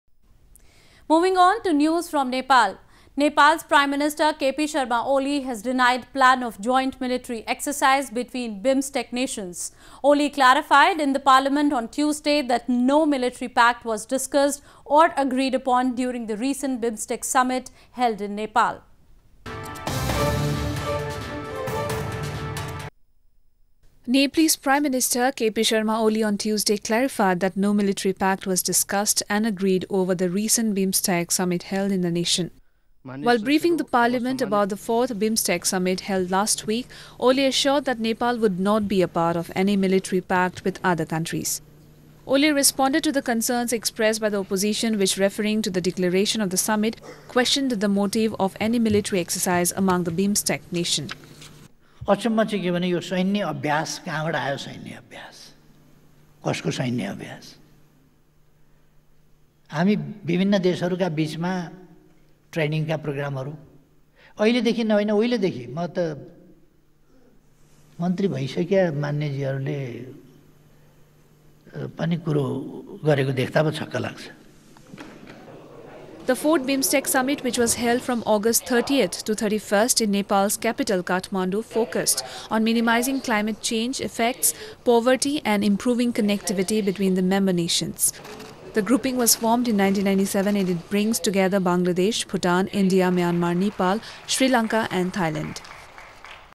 Nepalese Prime Minister Oli denies plan of joint military exercise between BIMSTEC nations
Nepal’s Prime Minister K.P Sharma Oli has denied plan of joint military exercise between BIMSTEC nations. Oli clarified in the parliament on Tuesday that no military pact was discussed or agreed during the recent BIMSTEC Summit held in Nepal.